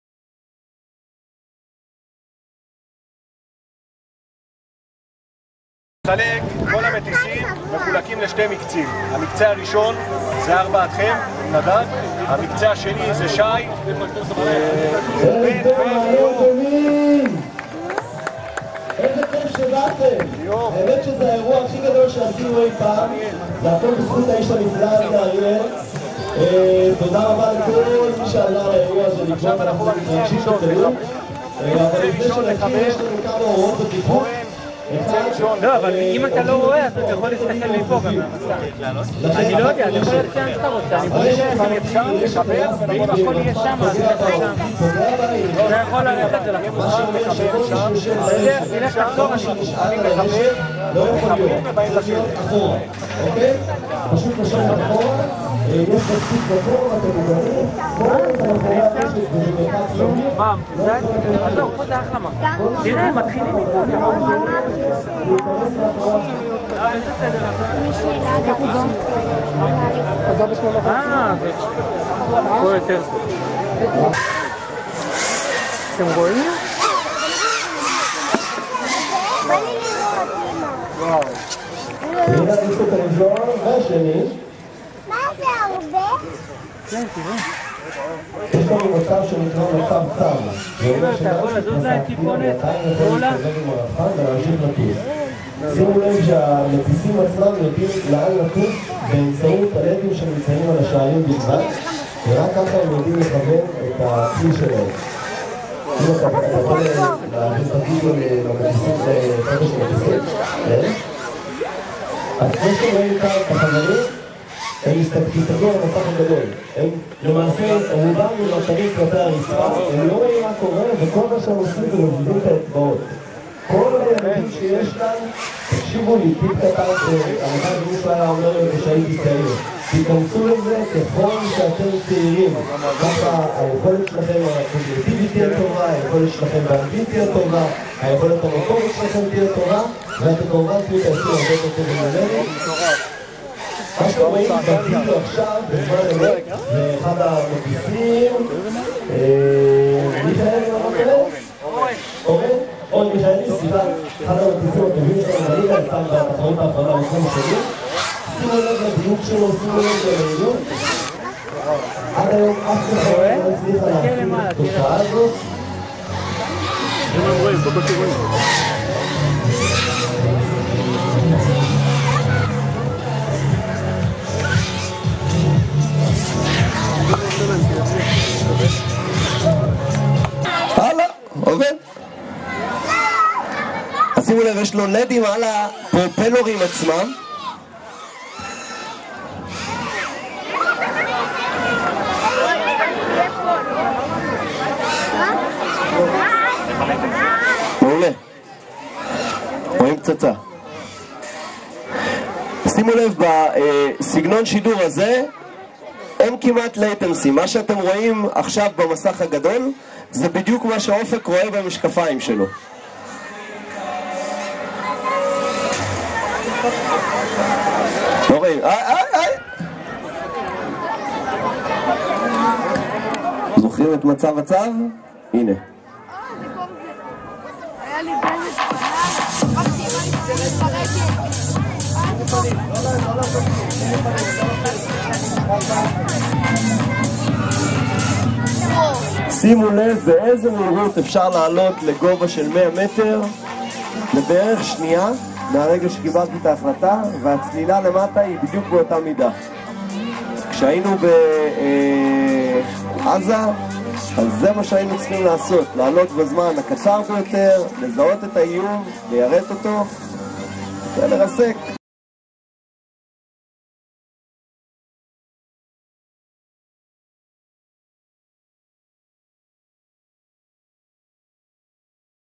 Drone Racing in Ma'ale Adumim - August 11, 2022
FPV Racing - Israel League,
Ma'ale Adumim Stadium